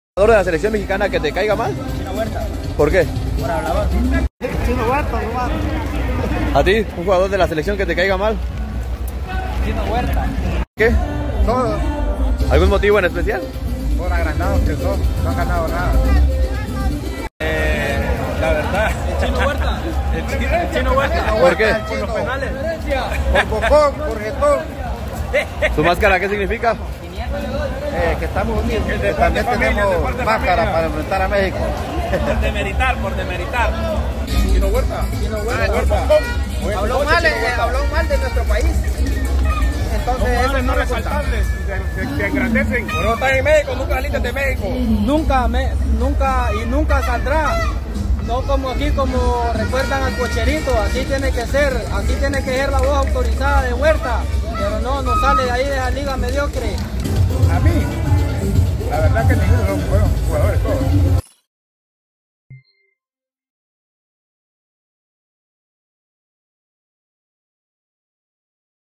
🇲🇽 ¿QUÉ JUGADOR DE MÉXICO ES EL QUE PEOR TE CAE? 😯 Esto contestaron los hondureños 👀
Hondureños aseguran que no quieren al jugador de Pumas por "hablador" 👀

📹 Sebastián García I EL UNIVERSAL